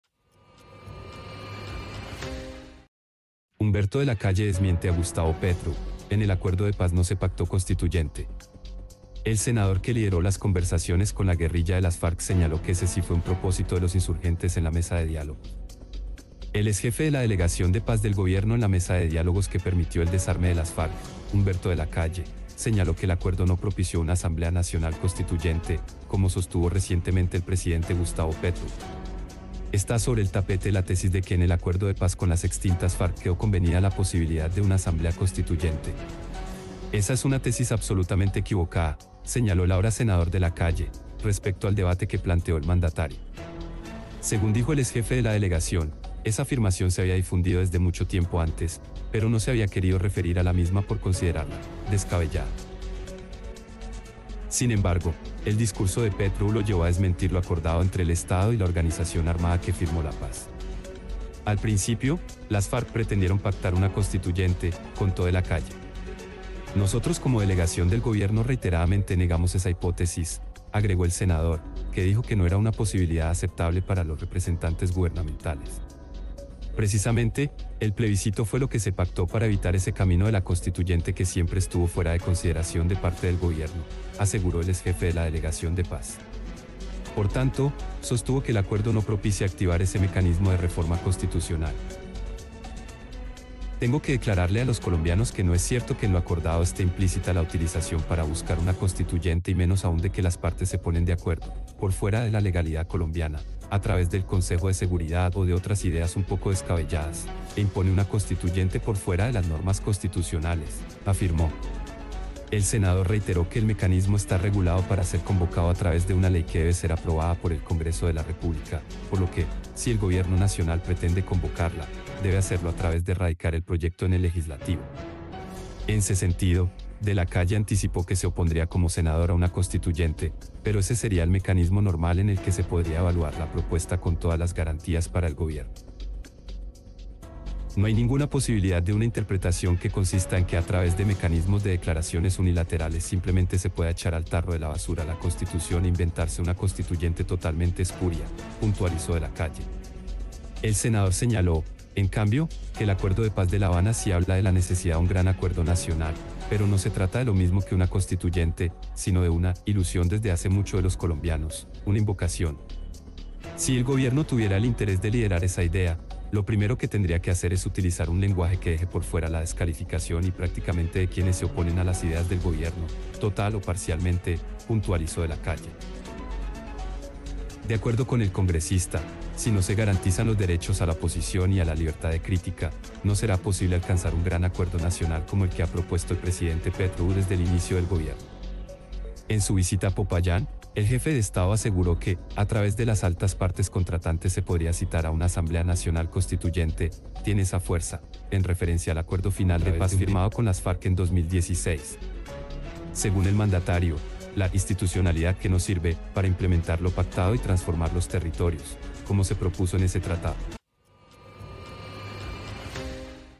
📌 HUMBERTO DE LA CALLE DESMIENTE A GUSTAVO PETRO
Humberto de la Calle desmiente a Gustavo Petro: “En el acuerdo de paz no se pactó Constituyente”
El senador que lideró las conversaciones con la guerrilla de las Farc señaló que ese sí fue un propósito de los insurgentes en la mesa de diálogos
INFOBAE